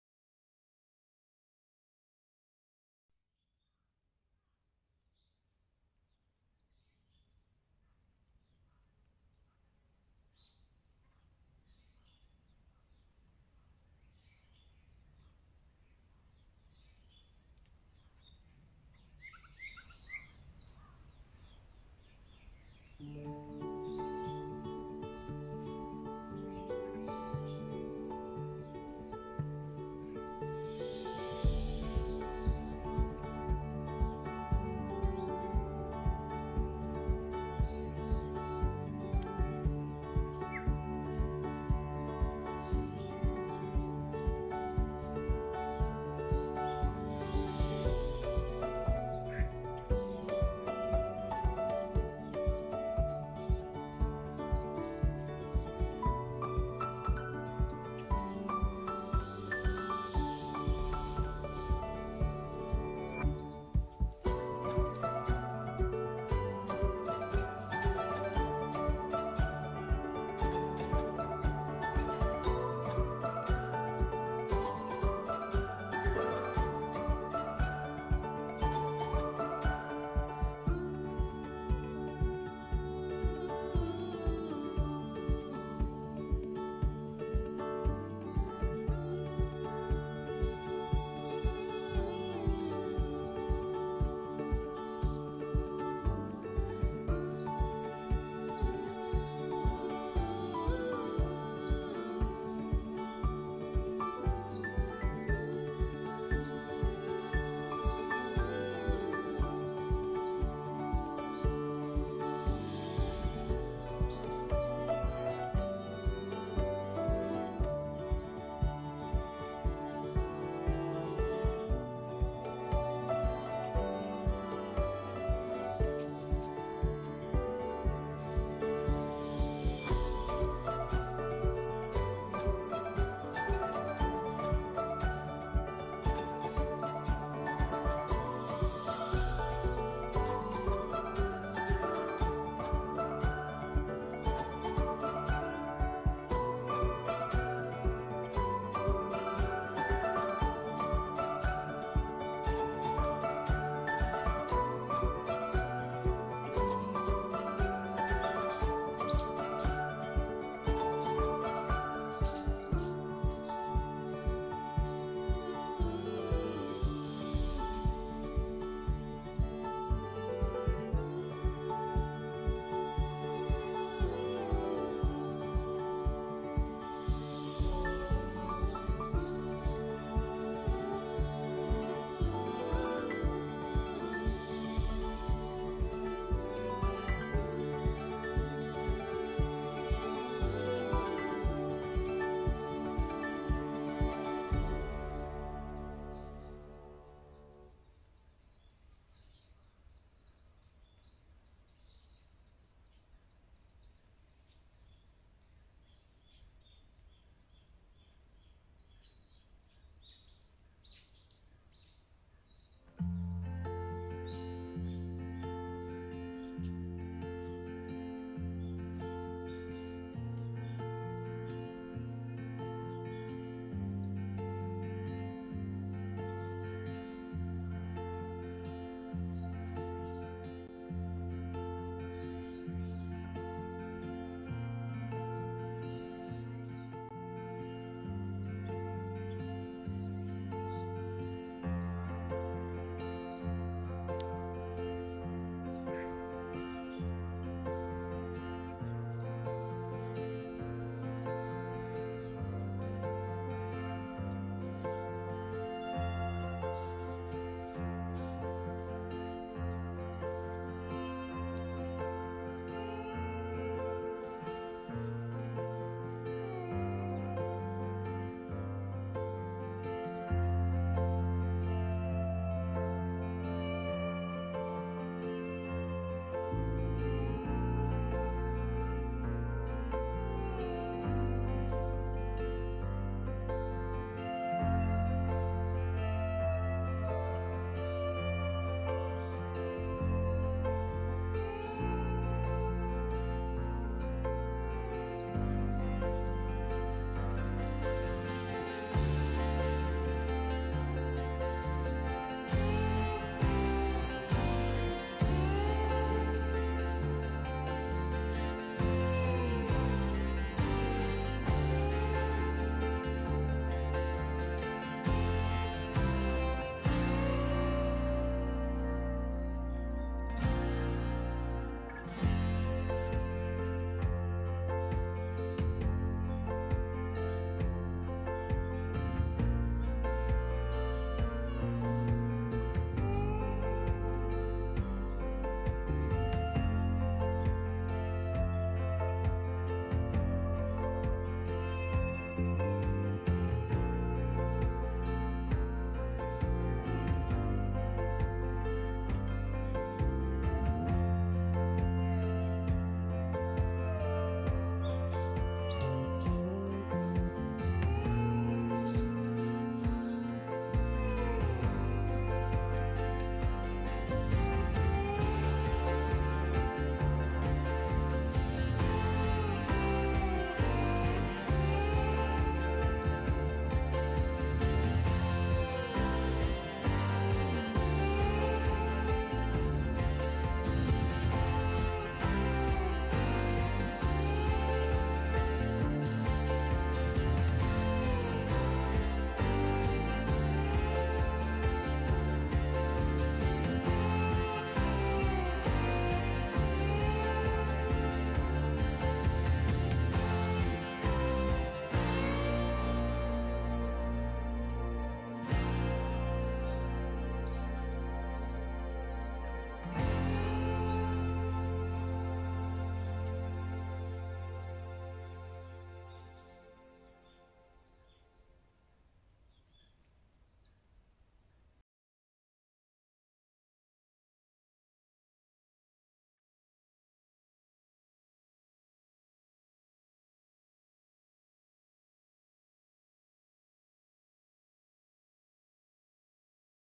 【Vlog】Happy morning! 虹の光と植物の力
越冬を頑張っている植物たち。
気持のよいある朝の動画です。